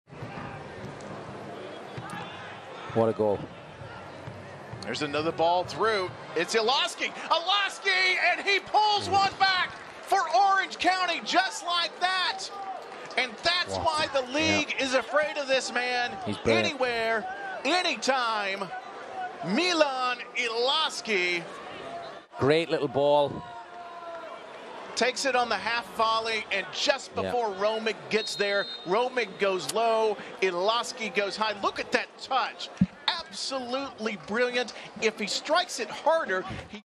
Milan Iloski with a Goal vs. Memphis 901 FC
Milan Iloski (Orange County SC) with a Goal vs. Memphis 901 FC, 07/22/2023